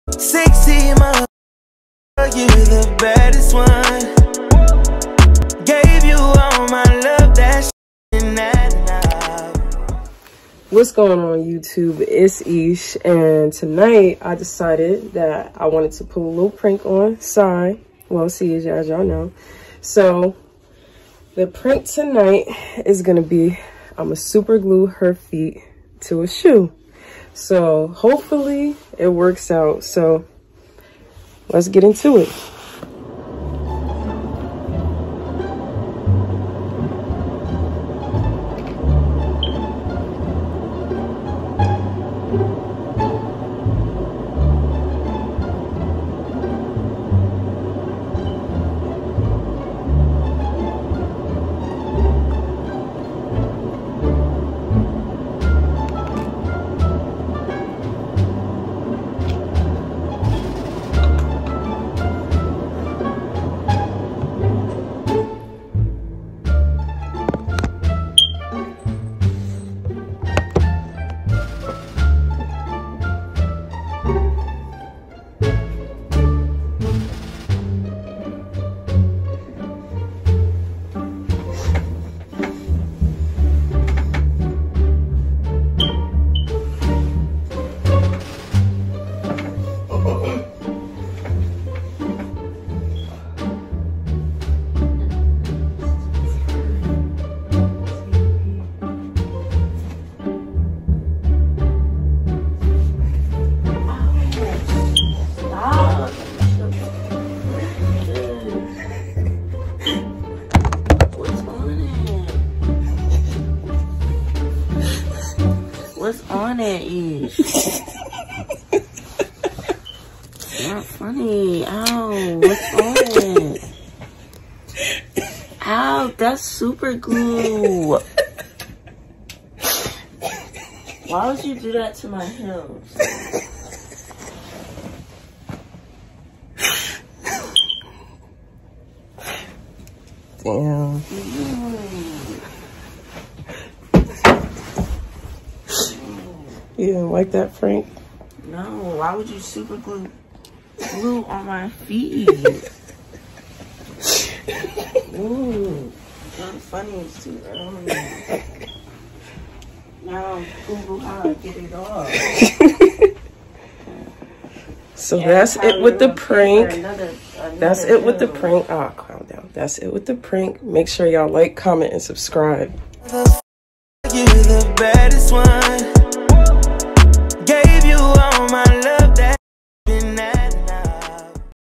SUPER GLUING HEELS TO HER FEET WHILE SHE SLEEPS!!!! *VLOGMAS DAY 1*
Thought I'd start vlogmas off with with a little prank lol.